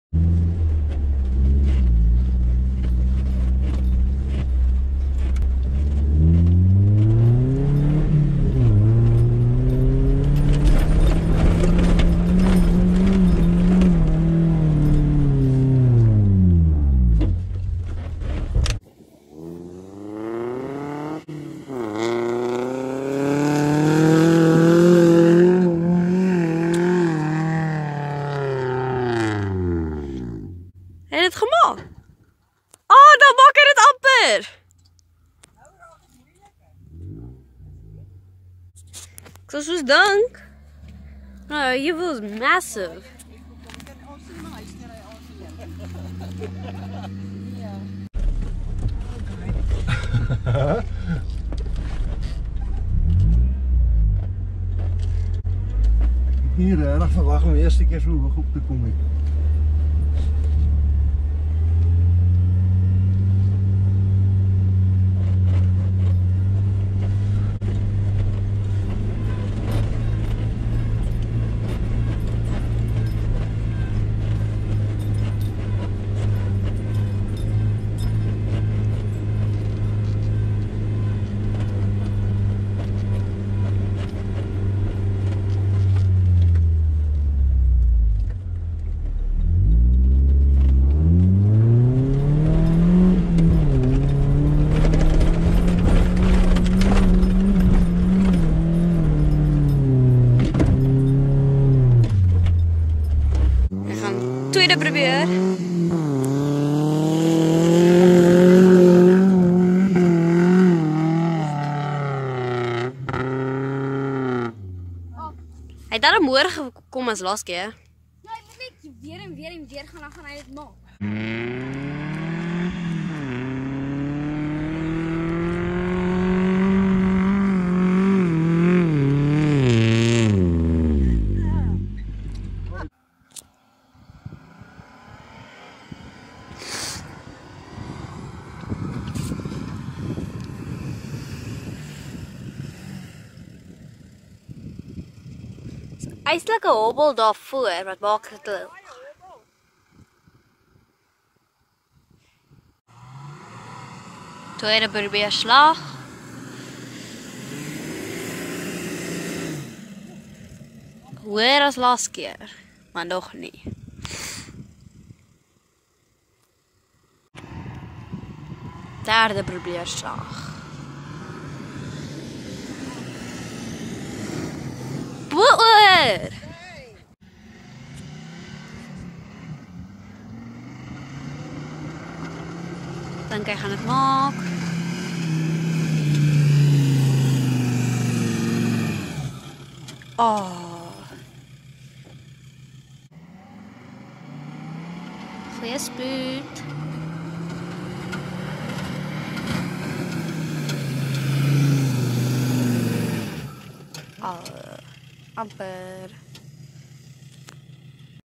Nissan 2 4, Amarok V6 and Ford Ranger on a dune.
We went to Amam Dune trails and tried out the 3 pickups on the Dunes. This video is just on the one slipface dune. Even the 2.8 GD6 and Pathfinder had its work cut out to do this dune.

the Ford did make the climb in the end , unfortunately we did not get it on video.